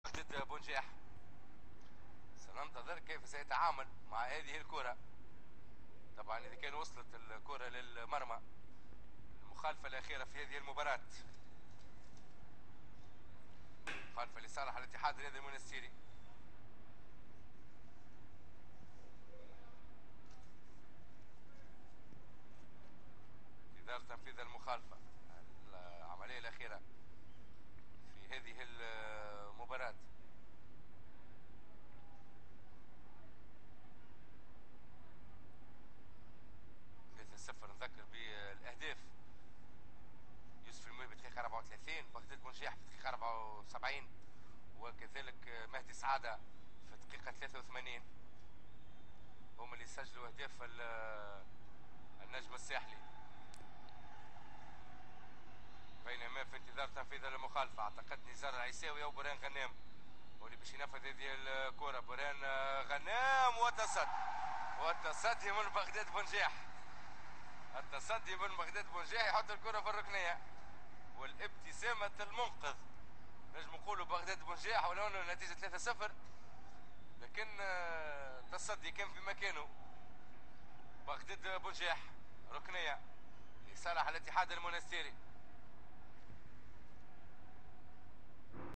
شاهد بغداد بونجاح لما لعب كحارس مرمى عندما كان لاعبا في نادي النجم الساحلي التونسي
بغداد بونجاح
حارس مرمى
النجم الساحلي التونسي